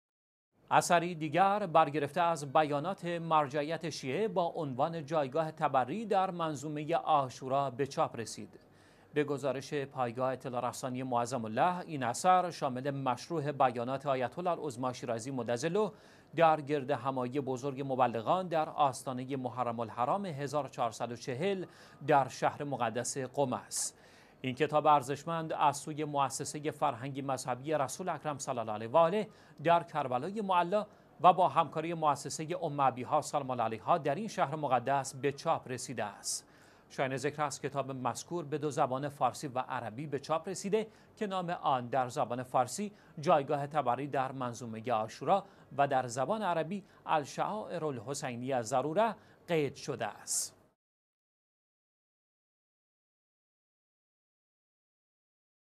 چاپ اثری دیگر از بیانات آیت الله العظمی شیرازی با عنوان «جایگاه تبری در منظومه عاشورا»
اثری دیگر برگرفته از بیانات مرجعیت شیعه، با عنوان «جایگاه تبری در منظومه عاشورا»، به چاپ رسید.

به گزارش پایگاه اطلاع رسانی معظم له، این اثر شامل مشروح بیانات آیت الله العظمی شیرازی (مدظله)، در گردهم آیی بزرگ مبلغان در آستانه محرم الحرام 1440 در شهر مقدس قم است.

این کتاب ارزشمند، از سوی موسسه فرهنگی ـ مذهبی «رسول اکرم صلی الله علیه وآله» در کربلای معلی،  و با همکاری موسسه «ام ابیها سلام الله علیها» در این شهر مقدس به چاپ رسیده است.

شایان ذکر است، کتاب مذکور به دو زبان فارسی و عربی به چاپ رسیده که نام آن در زبان فارسی «جایگاه تبری در منظومه عاشورا» و در زبان عربی «الشعائر الحسینیة ضرورة» قید شده است.

پنج شنبه - ۳ آبان ۱۳۹۷